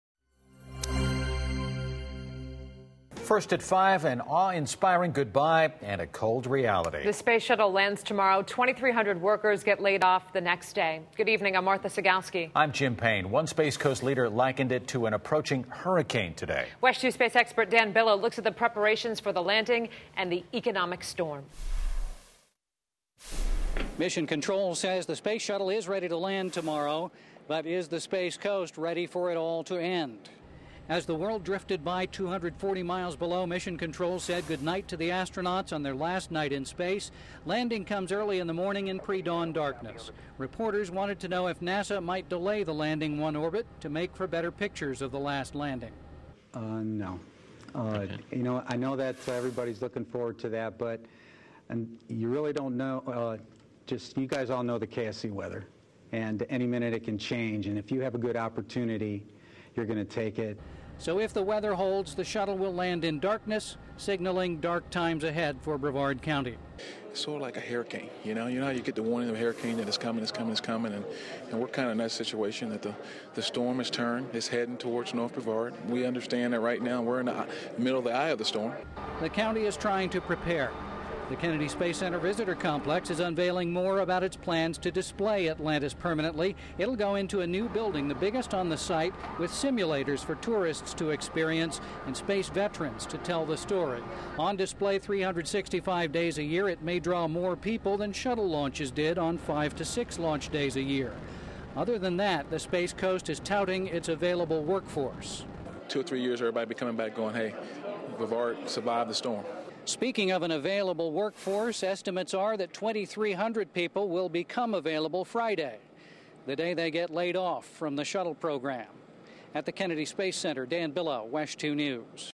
Atlantis Prepares For Landing
Space shuttle Atlantis prepares for its final landing.